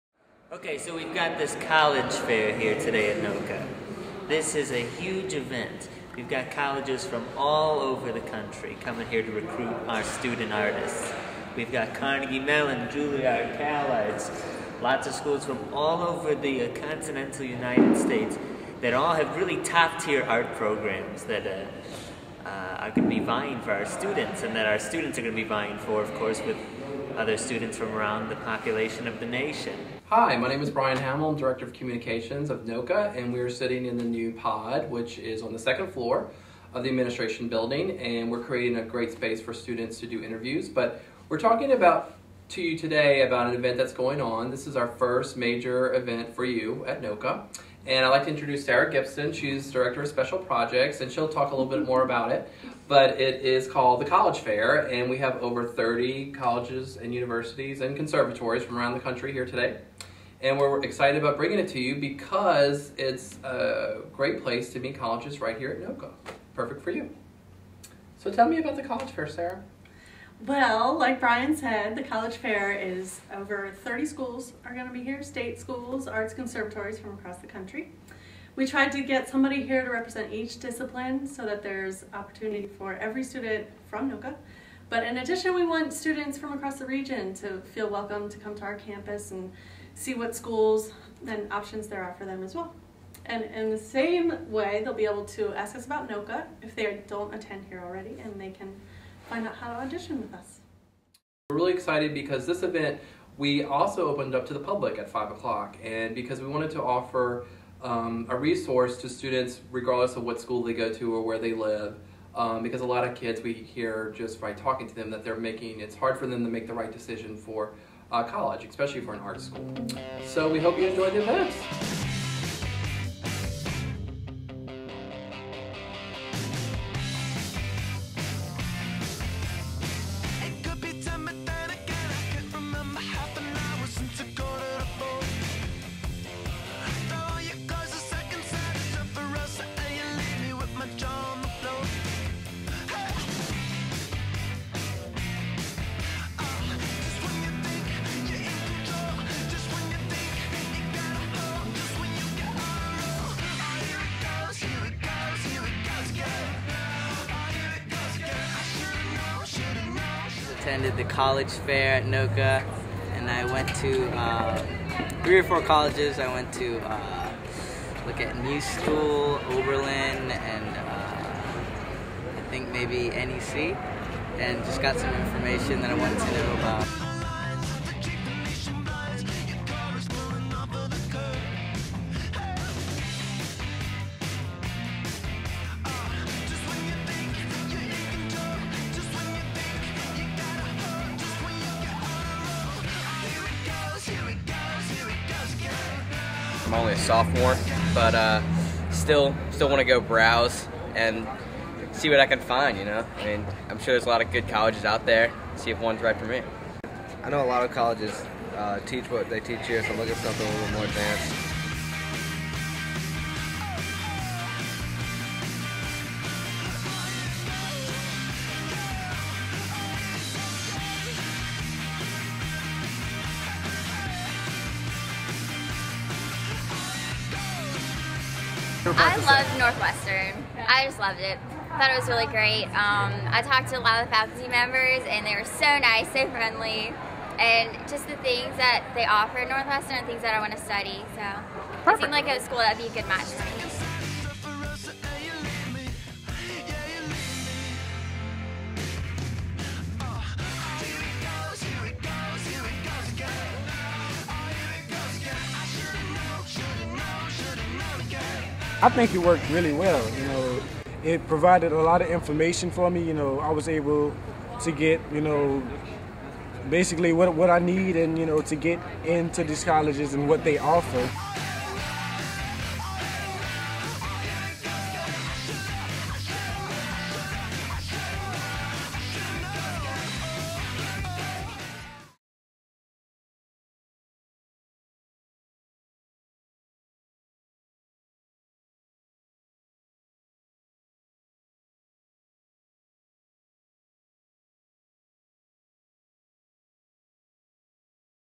2008 NOCCA College Fair
NOCCA opened its doors to students and the public on October 16th 2008 for its annual nationwide college fair.